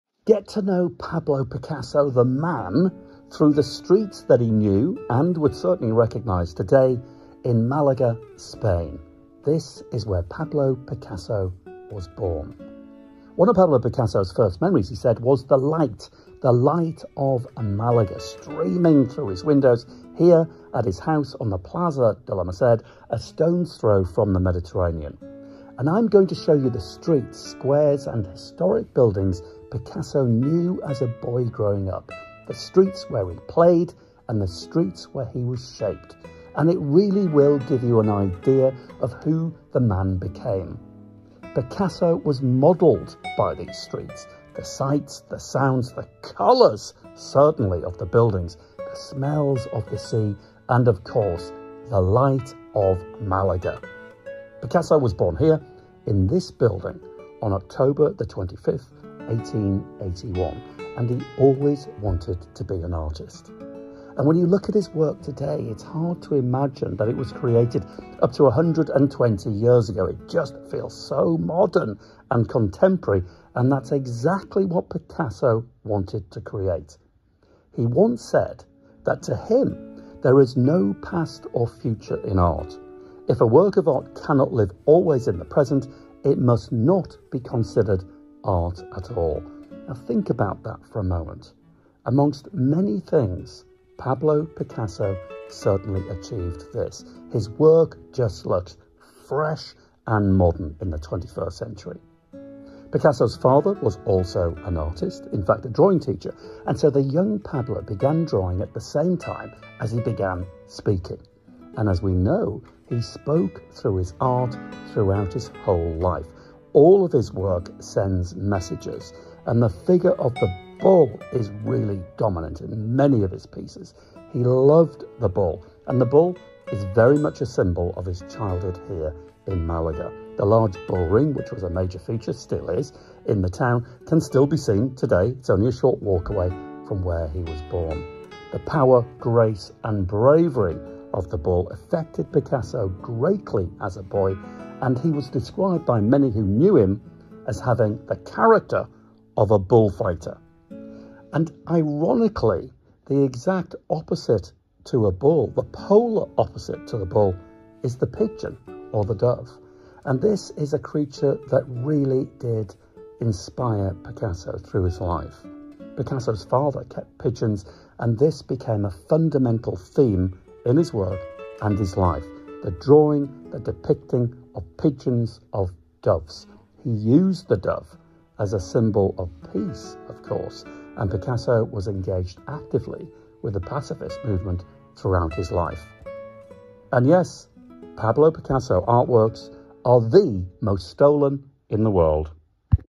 The MALAGA Back Streets and House where Picasso was born
I wanted to walk in the footsteps of Pablo Picasso. See the things that were familiar to him when he was growing up in Malaga, Spain. So, I visited the house where he was born (it’s now a museum) and I wandered around the squares and the tiny alleyways around his old house. All of what you see in this video would be recognisable to Picasso. You’ll get a sense of what he knew, what was normal to him and see the surroundings that inspired him throughout his life. These are the streets where Picasso played